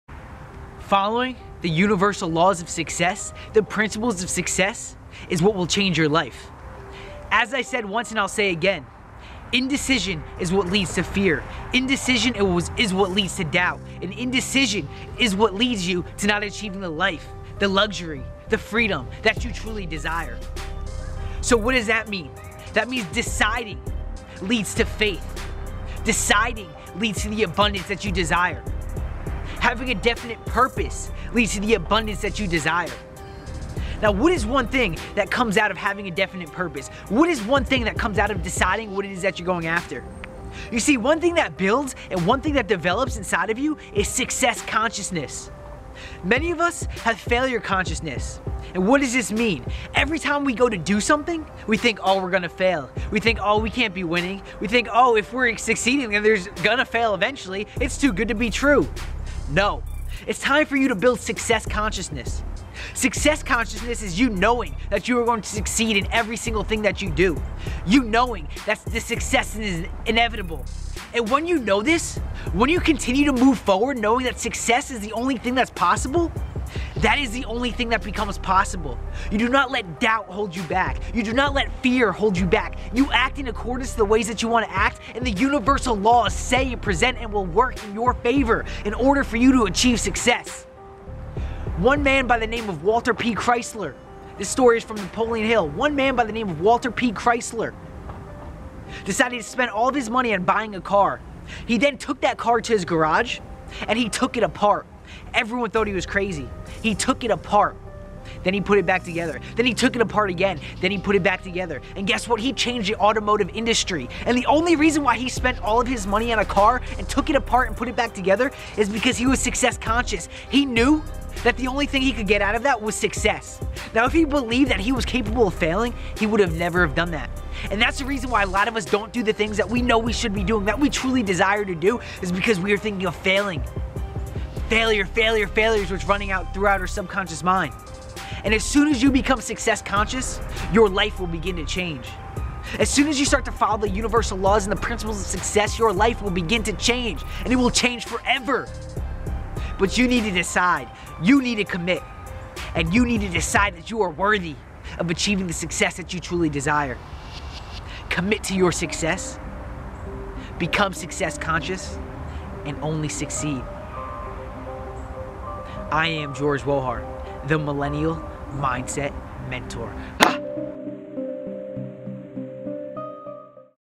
Become Success Conscious - Millennial Mindset Mentor
The One Quality That All Of The Greatest Men In History Follow is Success Consciousness.. Decide